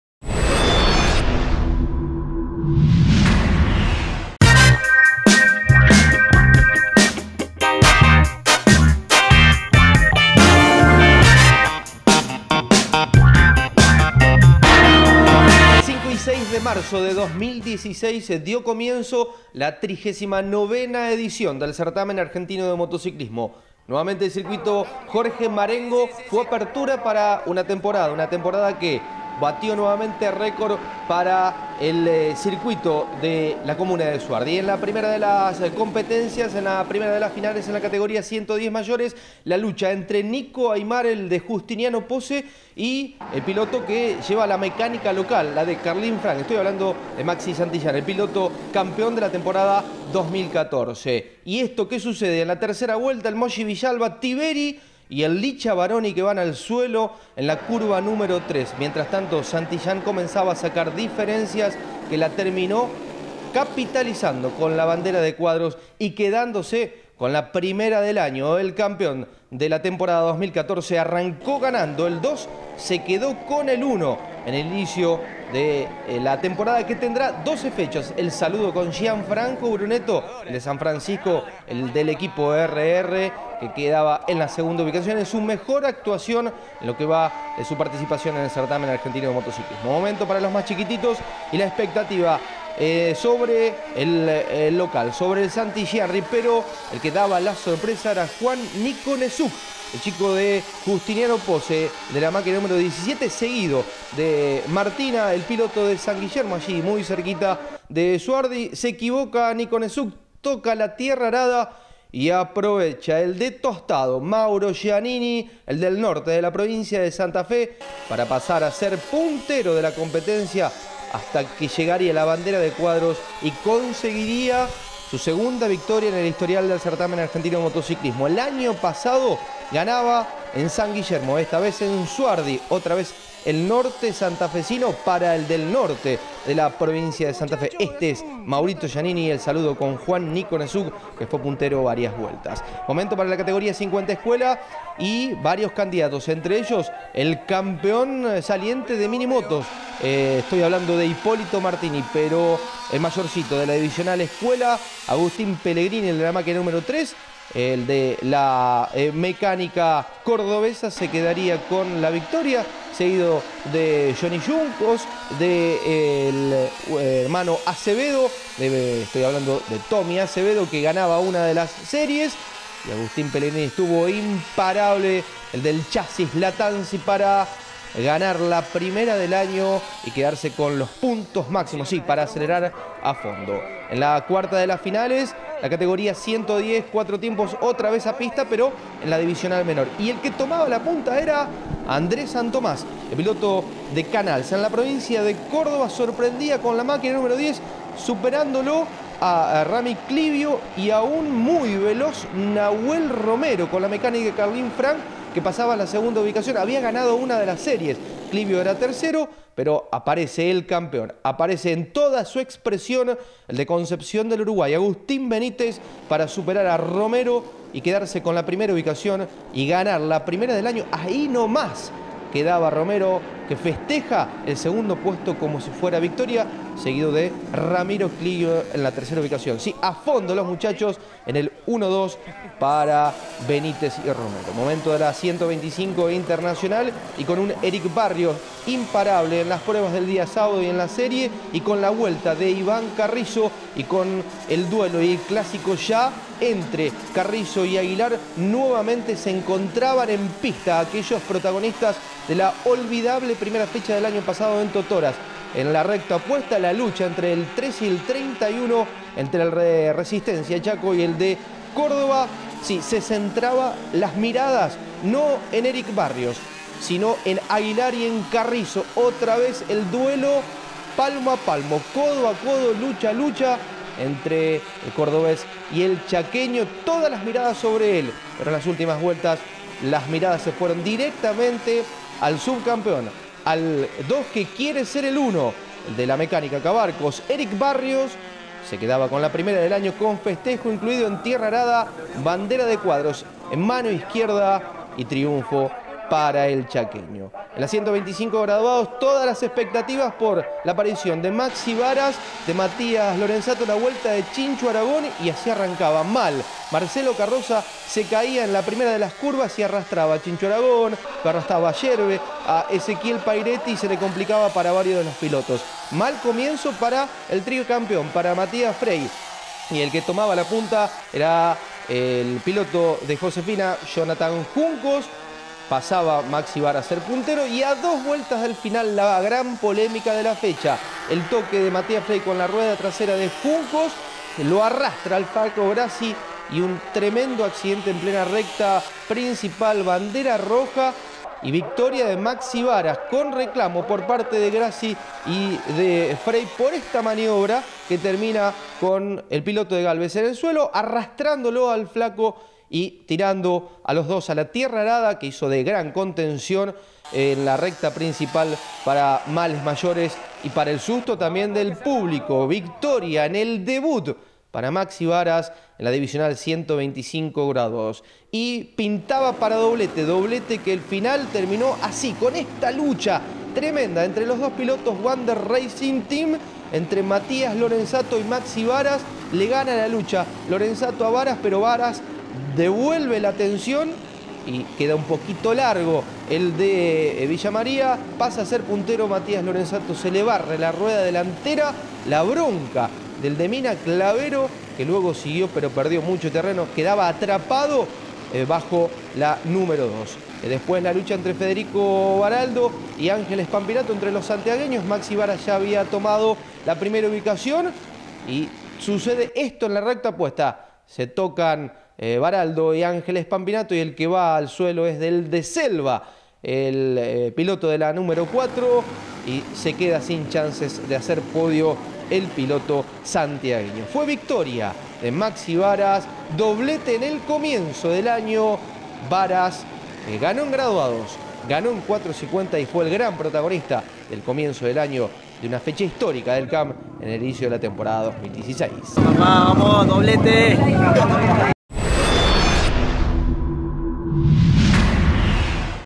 CAM Resumen #1 Fecha 2016 Suardi
Fecha Apertura 2016 del Certamen Argentino de Motociclismo - Campeonato 2016 - Circuito Jorge Marengo de Suardi, Santa Fe.-